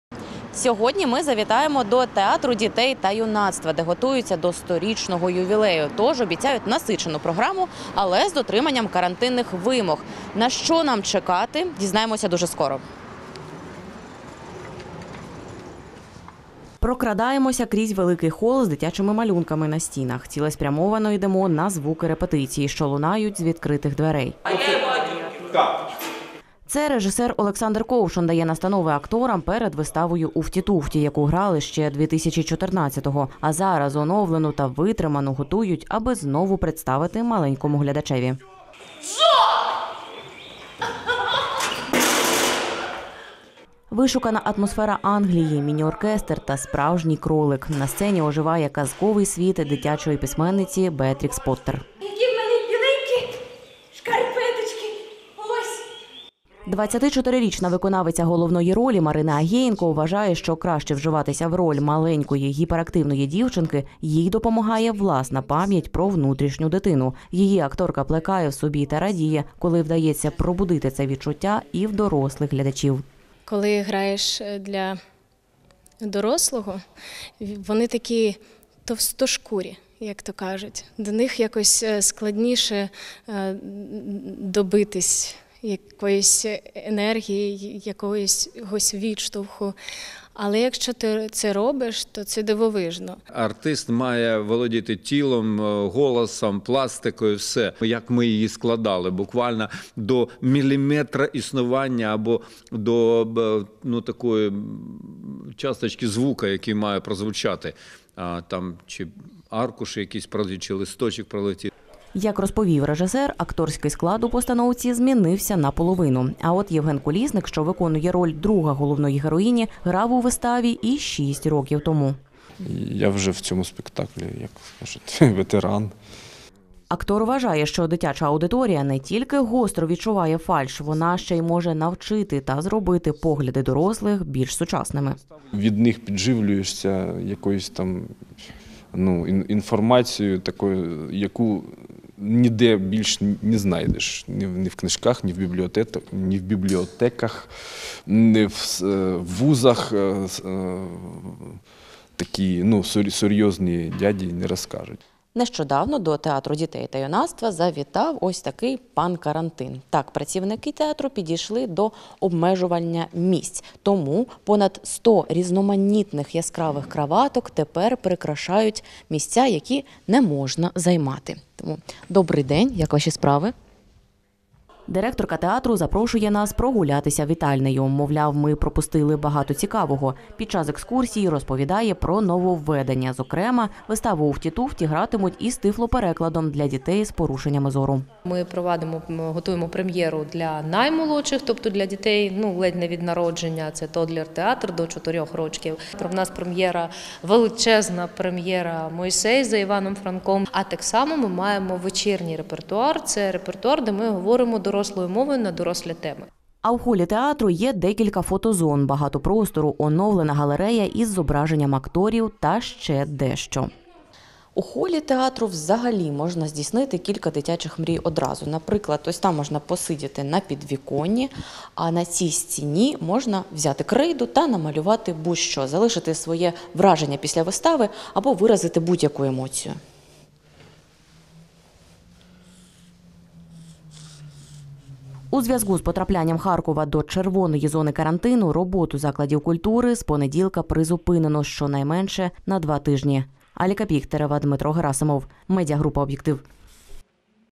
Театр для дітей та юнацтва оновився до 100-річного ювілею
Місце, де власні дитячі мрії можуть здійснити дорослі. У Харківському театрі для дітей та юнацтва можна сидіти на підвіконні, малювати на стінах, робити креативні фотознімки та, звісно, дивитися вистави. У свій ювілейний сотий сезон трупа входить із постановками для глядачів віком від 2 років. Дорослі ж можуть побачити вистави на серйозні теми.